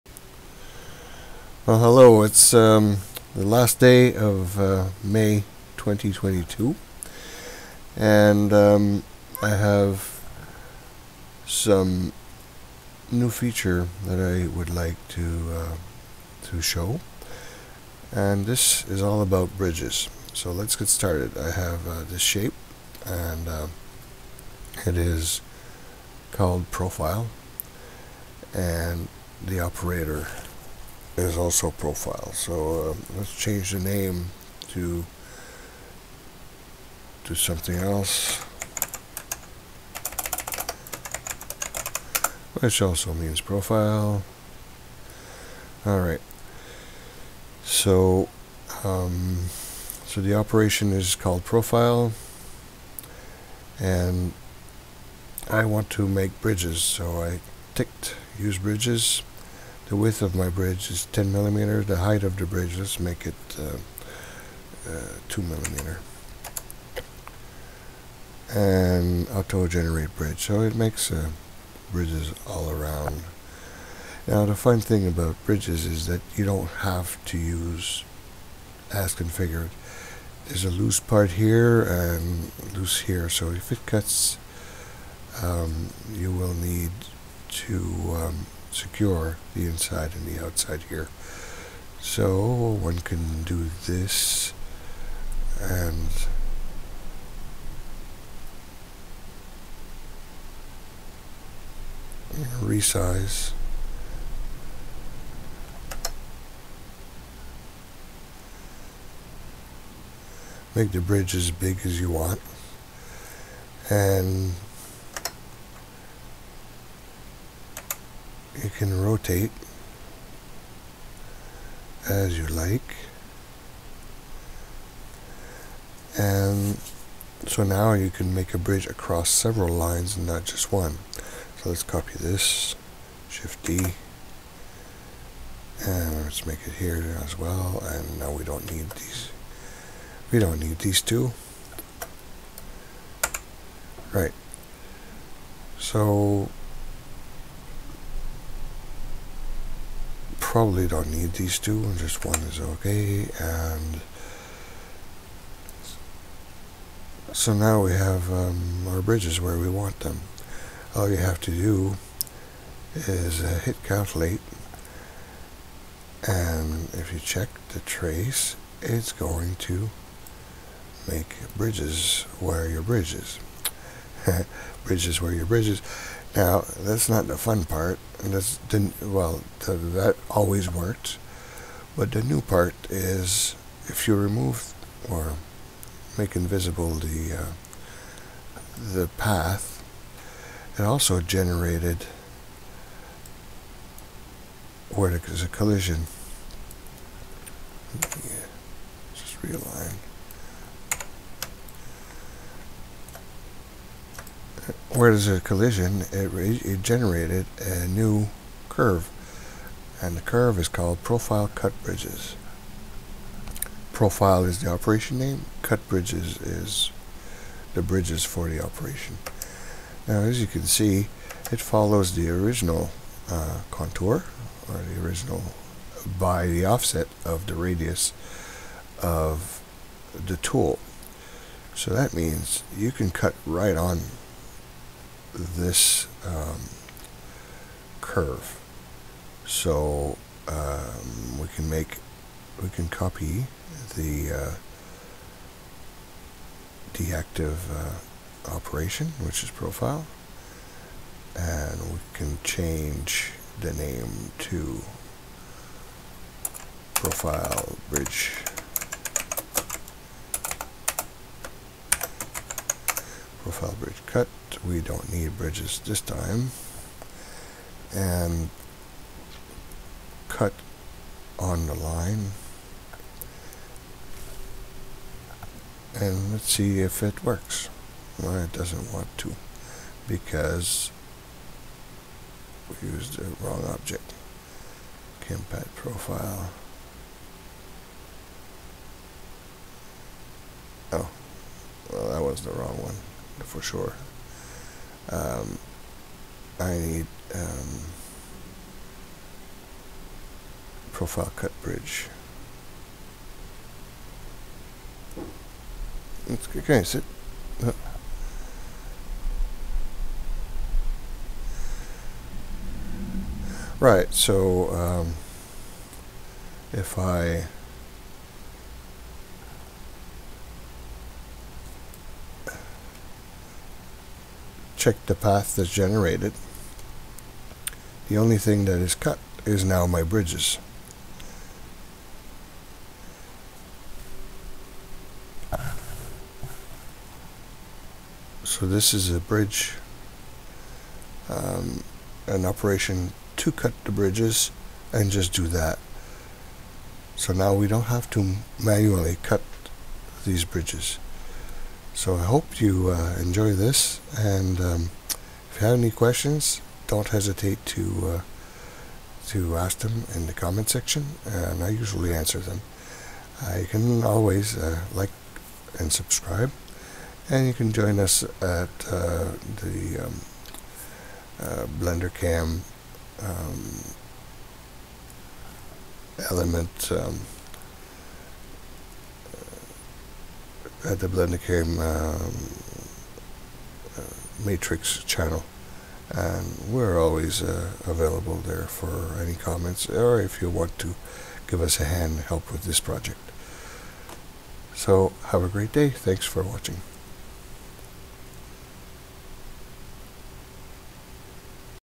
fabex cnc: Fun with bridges - cutting bridges
fabex cnc (formerly Blendercam)

I am the developer of this new bridge cut feature.

You can join us for a chat at matrix
if you want to chat.
We are the contributors keeping fabex cnc (formerly blendercam) alive and well.